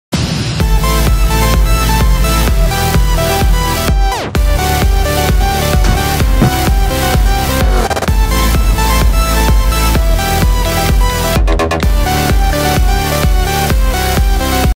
New Intro!
I got a suggestion for a intro and BAM here it is! Tell me what you guys think in the comments! Need feedback!!!